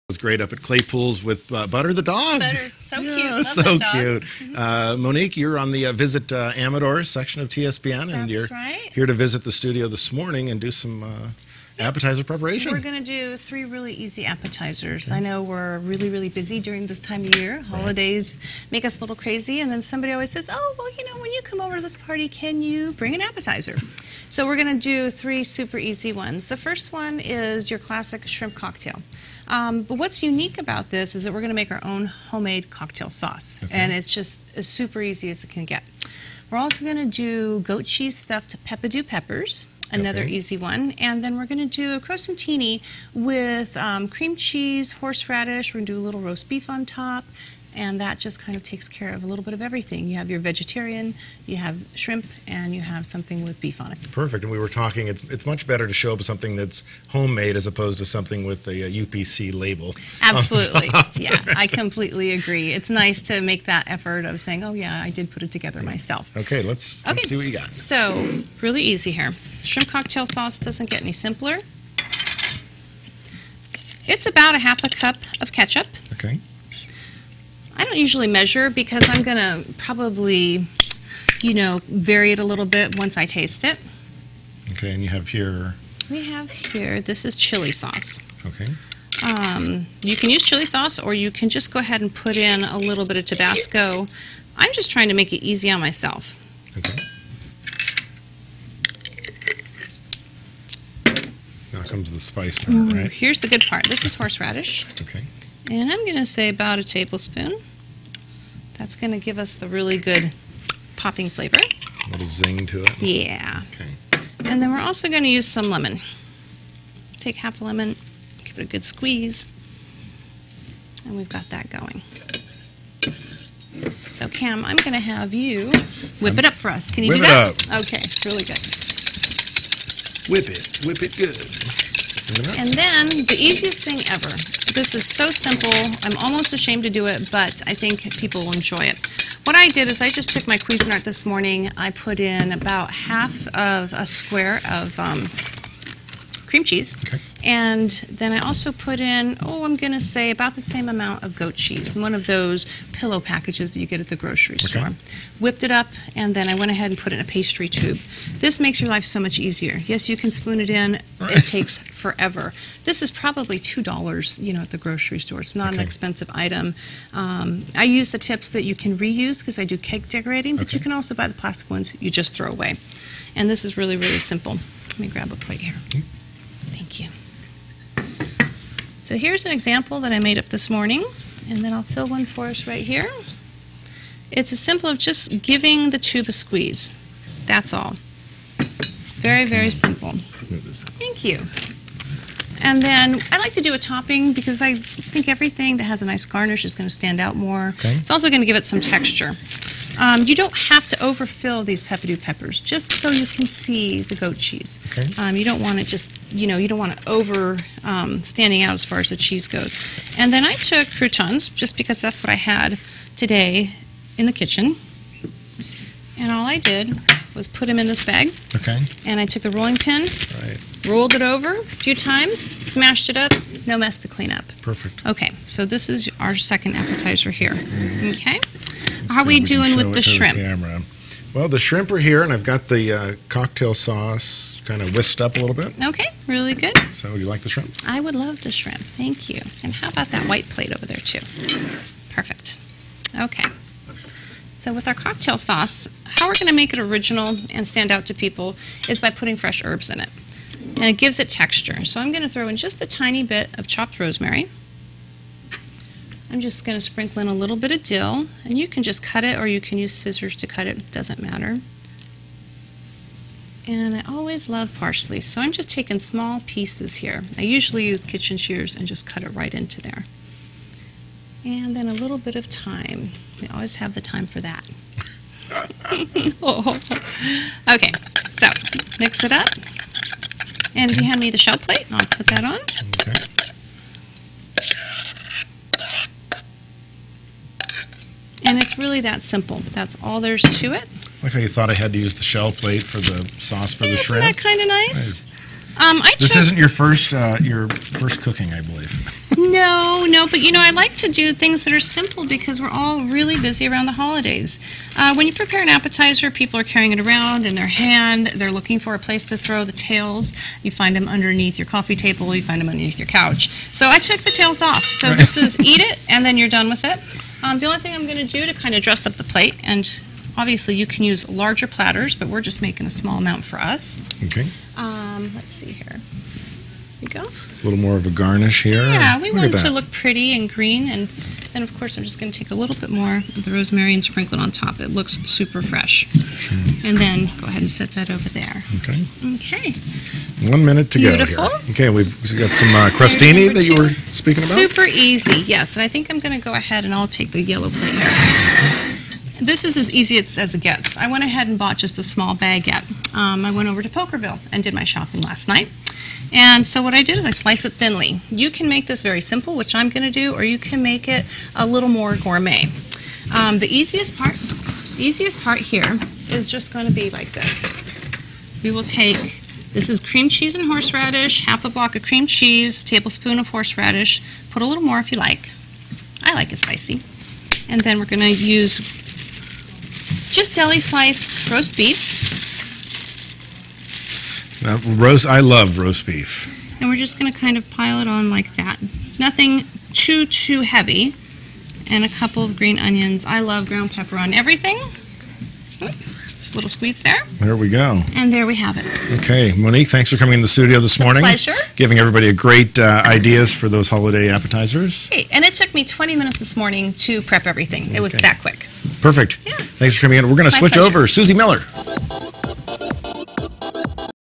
AM Live 12-7-12 - Holiday Party Snacks
Local chef Monique Graziadei stops by the AM Live set to cook up three wonderful appetizers that you can bring to any holiday party.